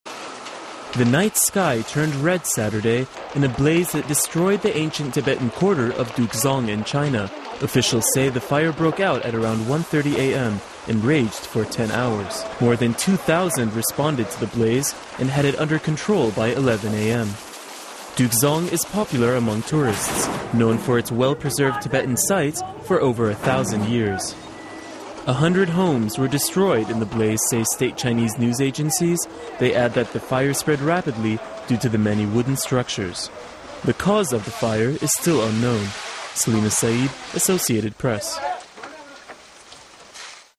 Ancient Tibetan Town in China Destroyed by Fire
A 10-hour raging inferno razed ancient Tibetan town Dukezong in China, a popular tourist area. Over 2,000 responders brought the fire under control by 11 a.m. (Jan. 11)